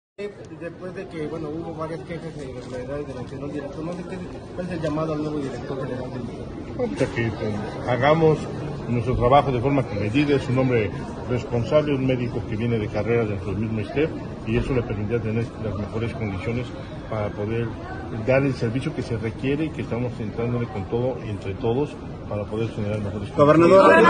Sergio Salomón pidió que se haga un trabajo con medida en la nueva dirección general del ISSSTEP
El gobernador de Puebla, Sergio Salomón pidió que se haga un trabajo con medida y de manera responsable en la nueva dirección general del ISSSTEP. Con el objetivo de generar mejores condiciones de servicio.